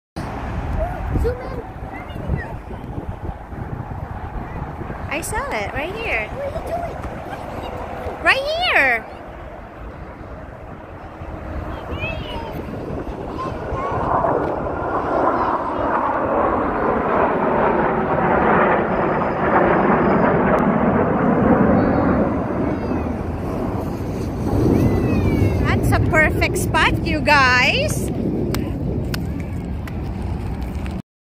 Blue Angels Flyby
Blue Angels Fly Over Chicago  to salute to frontline Covid-19 responders